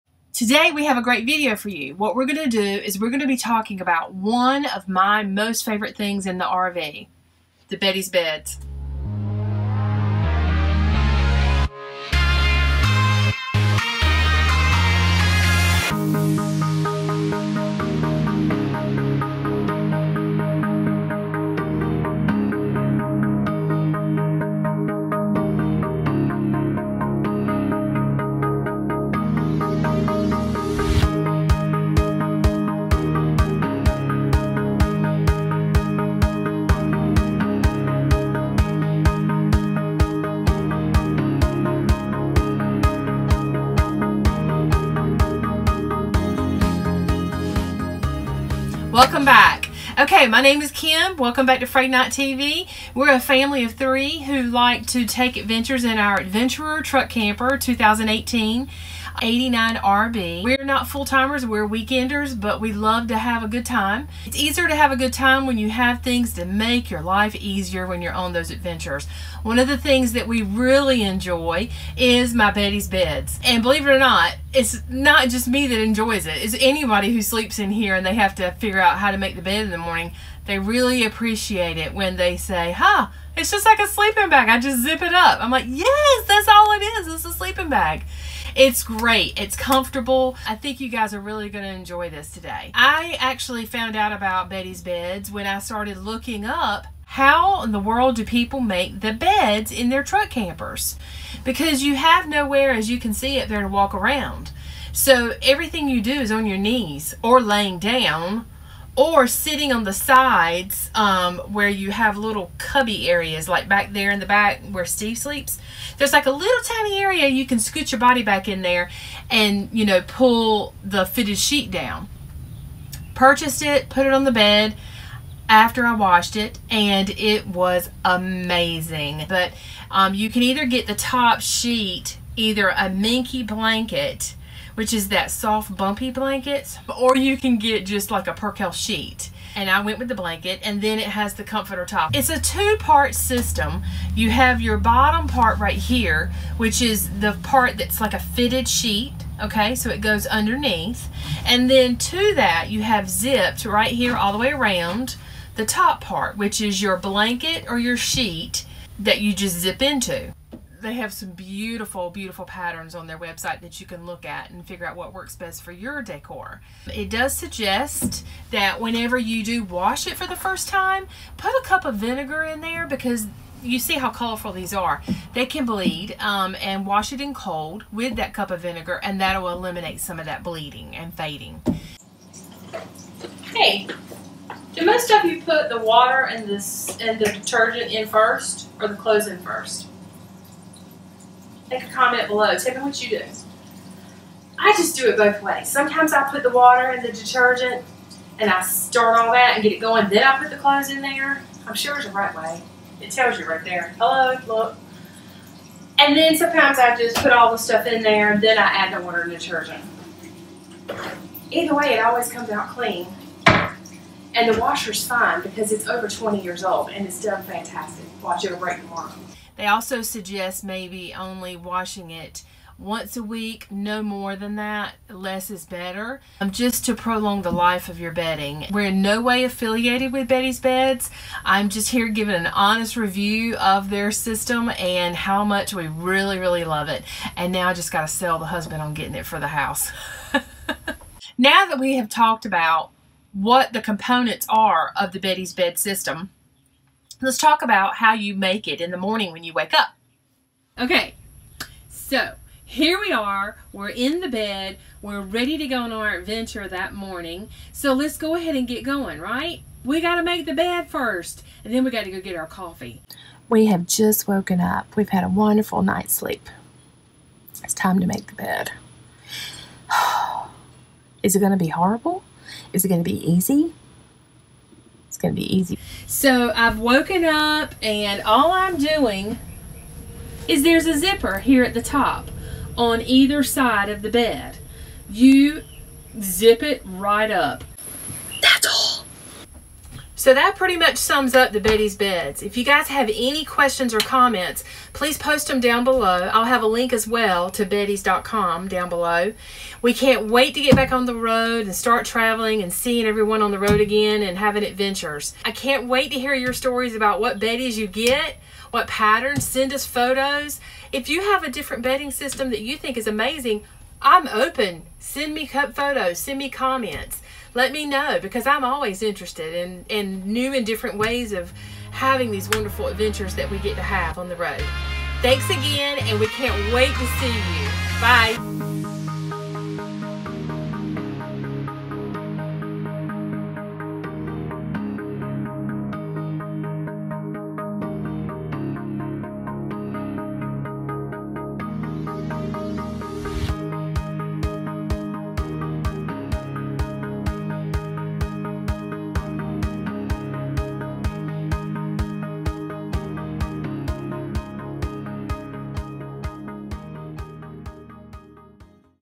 Beddy's Bedding
Today I will show you guys one of my favorite things about our RV Beddy's Bed!!! Its an amazing bedding for any size bed either in your home or in our case in our Truck Camper It can be so frustrating making the bed in a tight space like an RV and today we will show you how easy it really can be with Beddy's Beds!